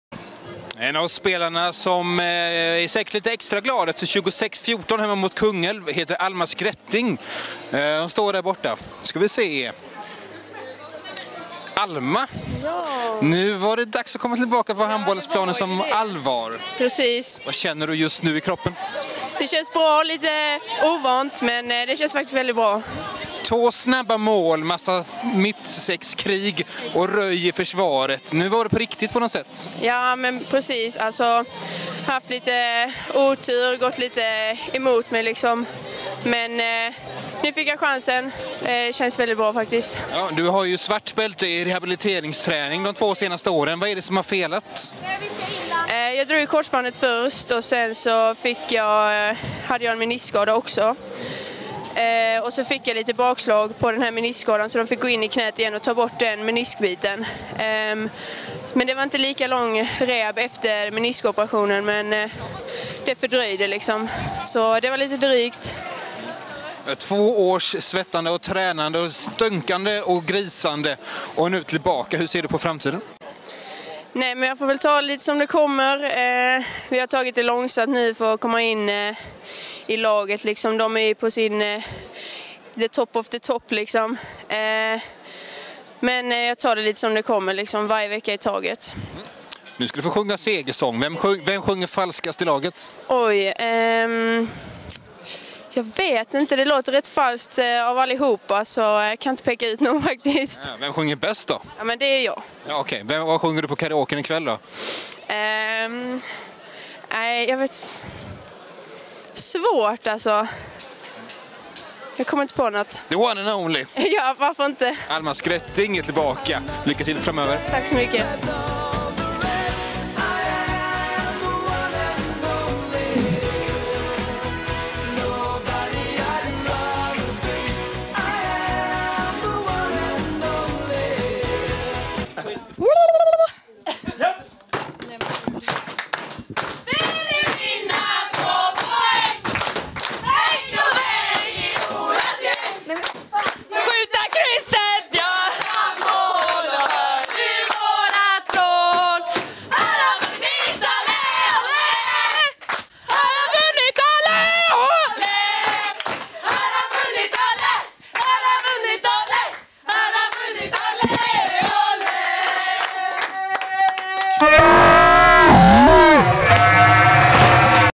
Höör har vunnit olé olé
Alma Skretting is back - och seger mot Kungälv med 26-14.
Tack alla som stöttad ei hallen och om någon har tips om en billig sångpedagog som kan arbeta under misäraktiga förhållanden, maila då till .